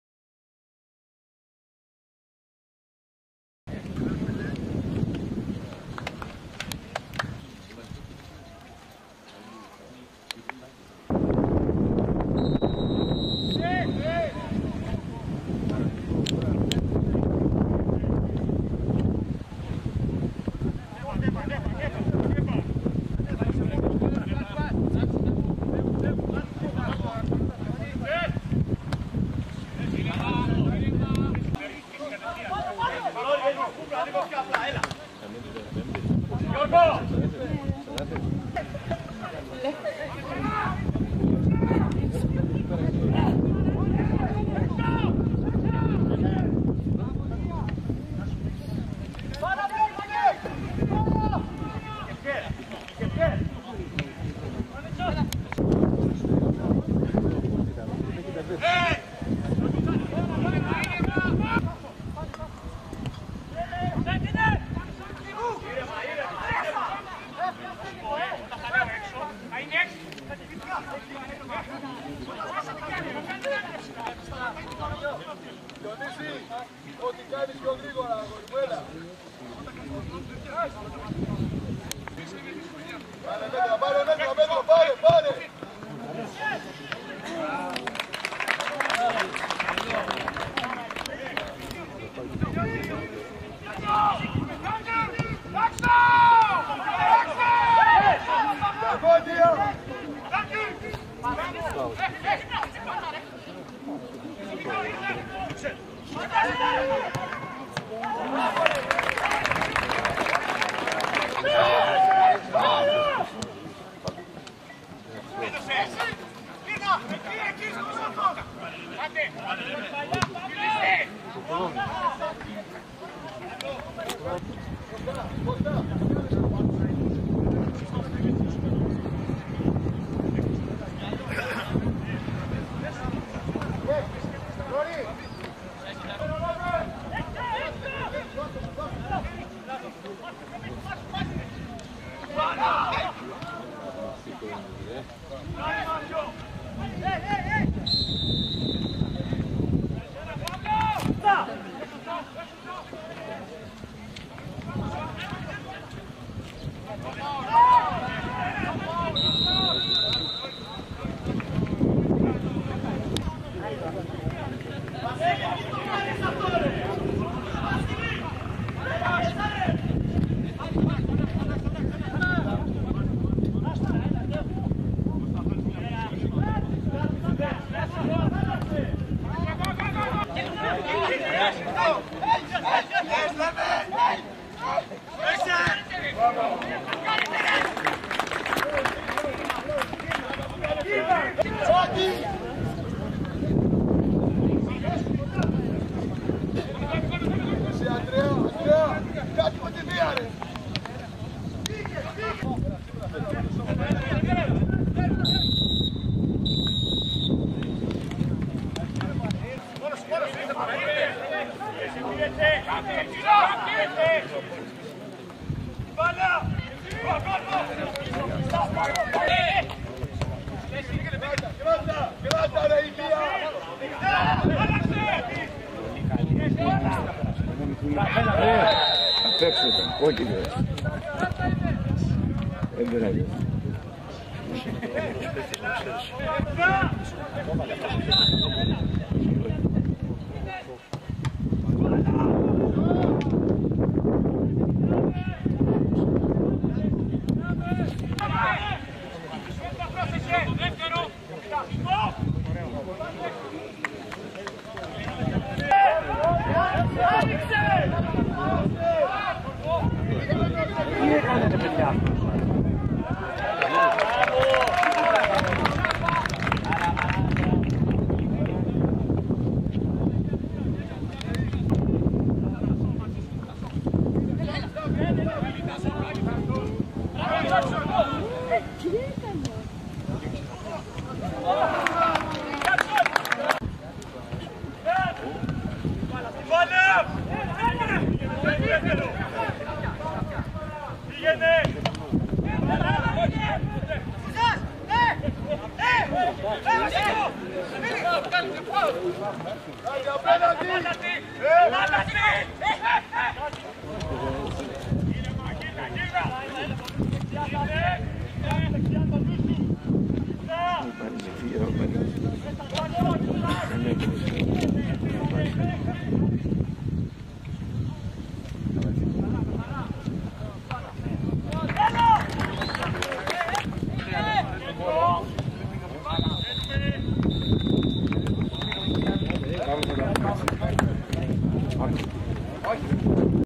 Φιλικό Τρίκαλα-Καλλιθέα 0-1 (09/08/2015)
(Video από κανάλι Goaltrikala Gr)
Καλλιθέα-Α΄ημίχρονο: Κατσούλης, Λεβέντης, Παγώνης, Νώνης, Πίκολο, Τεγούσης, Σταύρου, Μπέλης, Τσίπρας, Φράγκος, Προκς

Καλλιθέα-Β΄ ημίχρονο: Μιχελής, Συργιόπουλος, Μουχτάρης, Τσιώνης, Κατσικογιάννης, Σταμόπουλος, Μπούζας, Αρναρέλης, Σμυρλής, Ιγνατίδης, Τζάκσον.